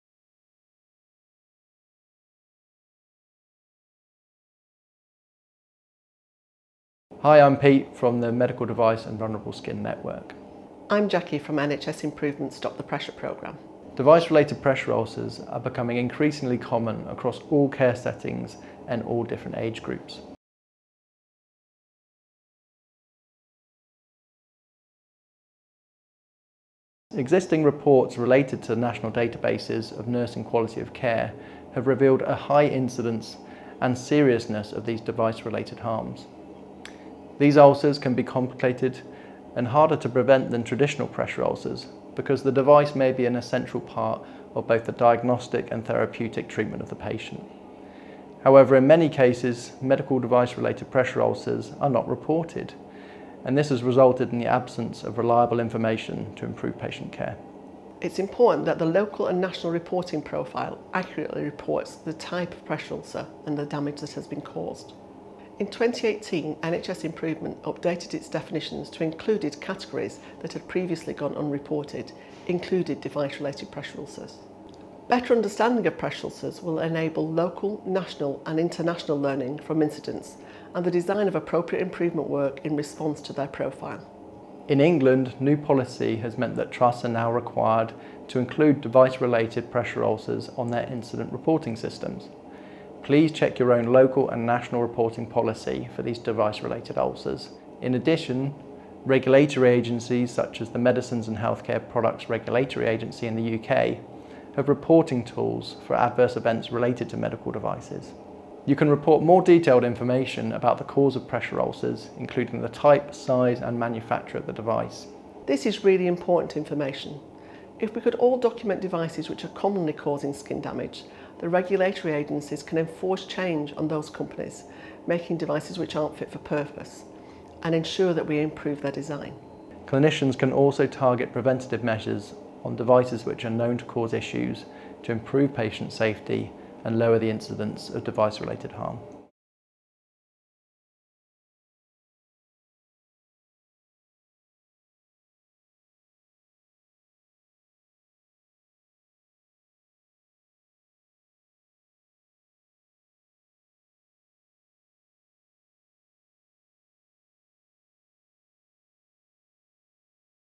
An overview of device related pressure ulcers
Peter Worsley from University of Southampton and Jacqui Fletcher from the Stop the Pressure Programme give an overview of why prevention of device related pressure ulcers is important on behalf of the NHS England and NHS Improvement Stop the Pressure Programme Critical Care Workstream.